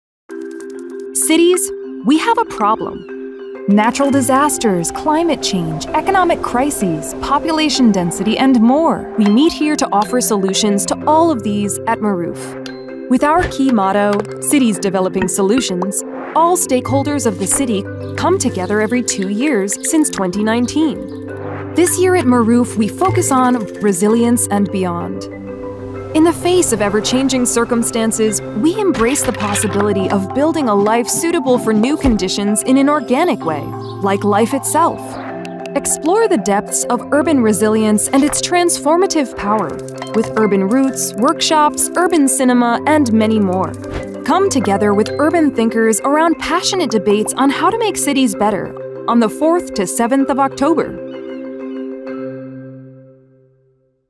MARUF23 invites you to “Resilience and Beyond”
Here comes MARUF23!
Marmara Urban Forum (MARUF23), which will be held in Istanbul on 4-7 October 2023, will meet with the citizens this year with the theme of Resilience and Beyond.
Everyone who thinks about the city and produces for the city is invited to MARUF23, which includes a variety of carefully prepared content from panels to movie screenings, from workshops to field trips.